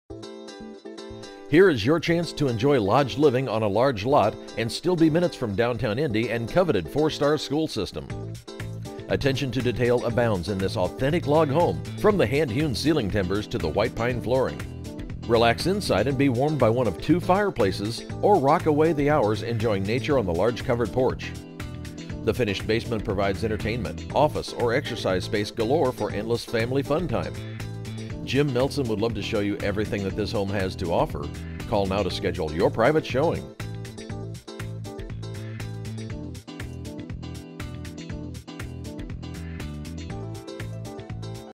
8698 County Road 650 Brownsburg, IN | MLS# 21447649
Jim Melson- |

LISTING INCLUDES TWO PARCELS, ACREAGE AND PROPERTY TAXES REPORTED ON LISTING SHEET ARE COMBINED.  MAIN LEVEL S.F. INCLUDES HEATED WORKSHOP IN DETACHED GARAGE.  3RD BEDROOM WALL HAS BEEN OPENED TO CREATE LARGER SPACE WITH 2ND BEDROOM-CURRENTLY PRESENTS AS A TWO BEDROOM HOME.  SELLER WILL RESTORE TO THREE BEDROOM BEFORE CLOSING IF REQUESTED.  HOT TUB, BASEMENT BAR, MINI-BARN, AND REFRIGERATORS CAN BE INCLUDED IN SALE WITH AN OUTSTANDING OFFER.  PRELIMINARY TITLE THROUGH SECURITY TITLE SERVICES.